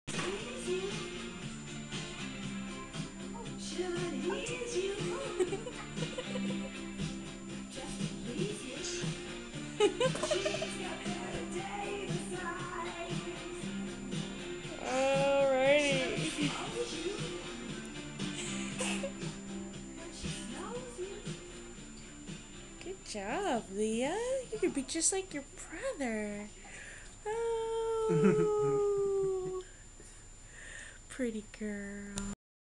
Lia "Dancing"
Leonard making Lia dance...just as he did Alexander around this age.